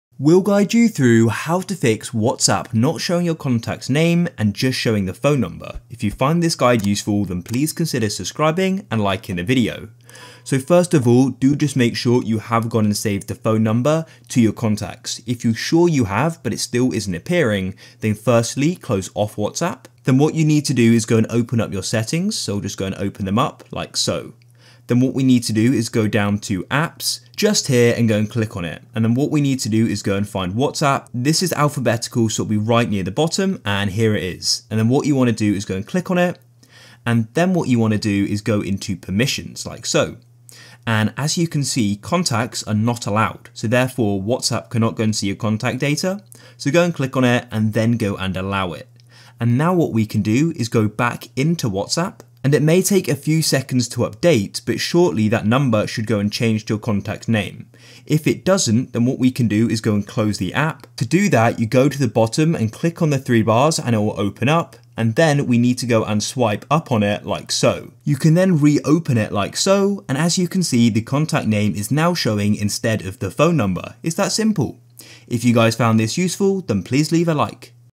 How To Fix WhatsApp Not Showing Contact Name Just Number On Android - Full Guide
Learn how to fix whatsapp not showing contact name just number on android in this video.

GuideRealm